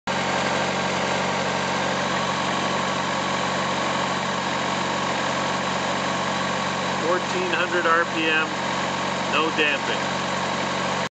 Fuse Relay Box on Engine 1400 rpm as built
Fuse Relay box vibration shown under a strobe light engine running at 1400 rpm (worst rpm for vibration).